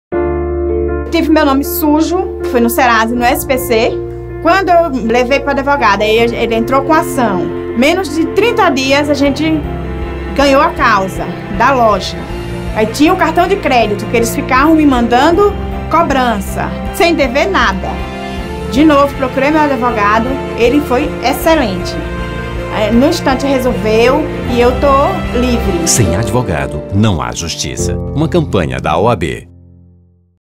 OAB-BA: Sem Advogado não há Justiça - Filme 2
Campanha de valorização do advogado realizada pela OAB da Bahia. Filme mostra depoimento de Rosângela do Santos, de Senhor do Bonfim, Bahia, contando que graças ao advogado ela conseguiu limpar seu nome no SPC e Serasa, que tinha sido colocado indevidamente.